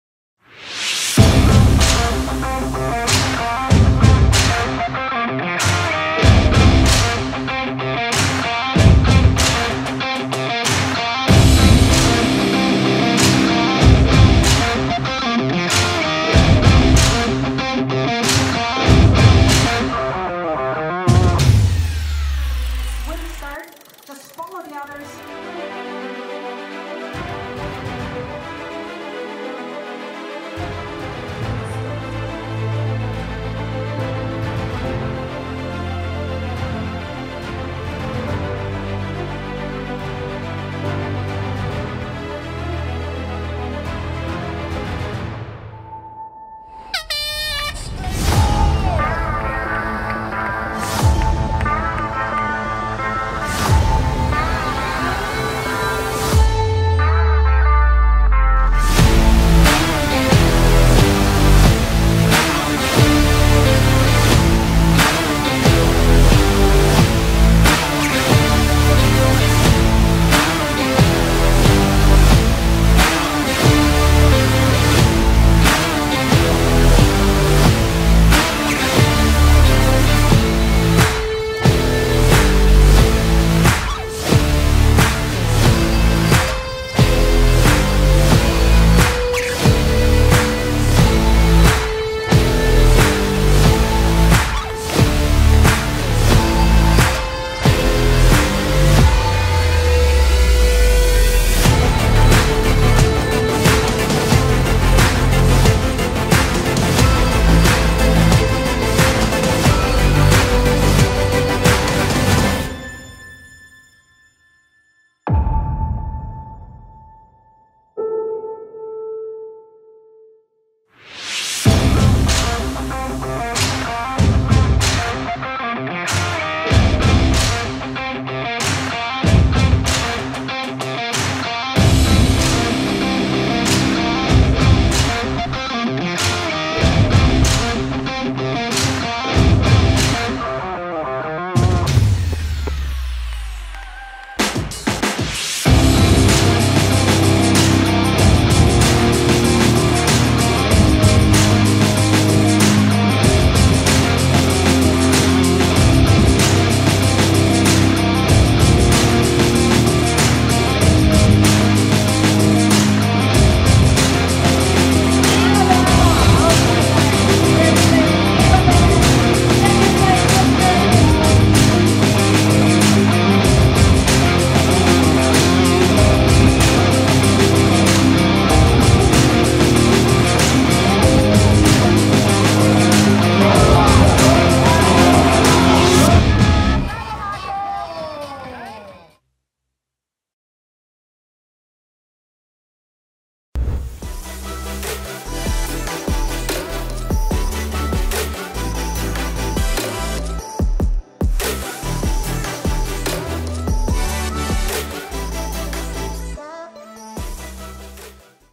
IRONMAN 70.3 Graz 2021 Race Movie
For the ultimate Austrian experience look no further than the IRONMAN 70.3 Graz - swim in the Schwarzlsee, bike through the Styrian countryside and run in the historic city centre of Graz that boasts views of the Schlossberg and many other impressive buildings